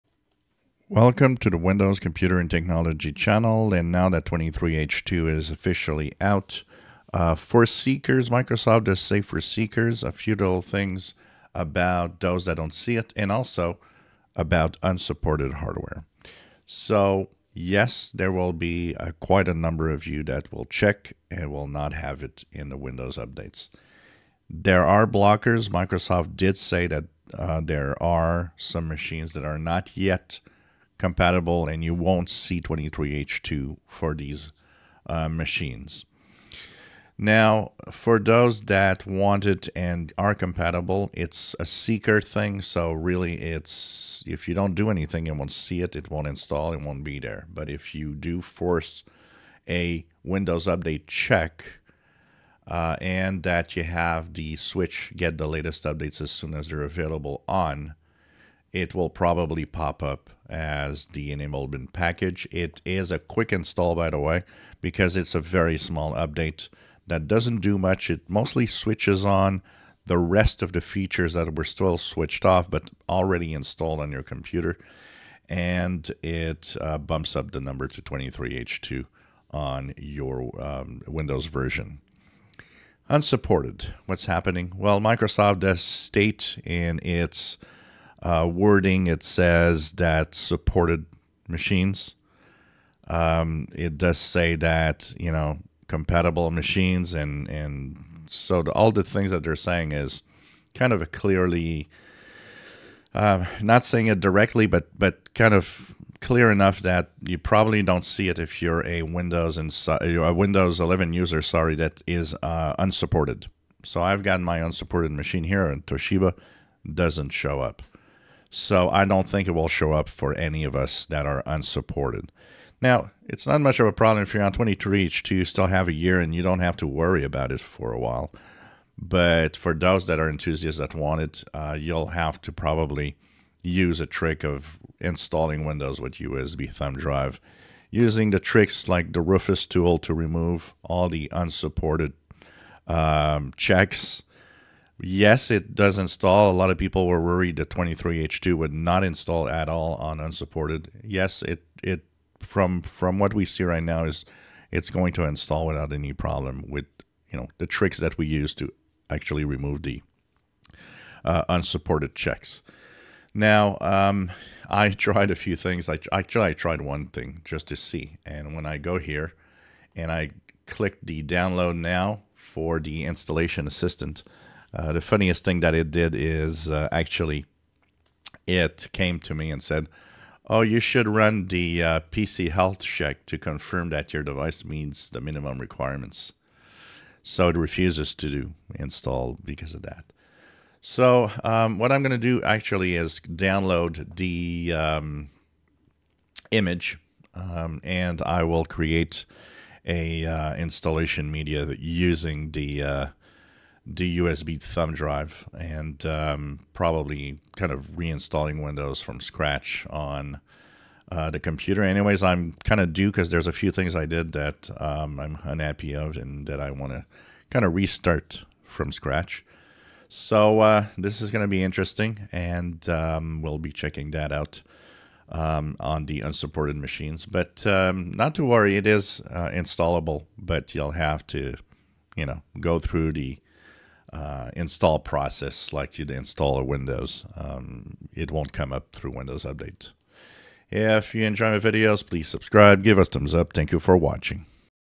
Windows 11 23H2 Seekers Compatible machines and unsupported machines
Not everyone will be offered 23H2 right away, unsupported will have to install Windows 11 23H2 as it will not show up in Windows update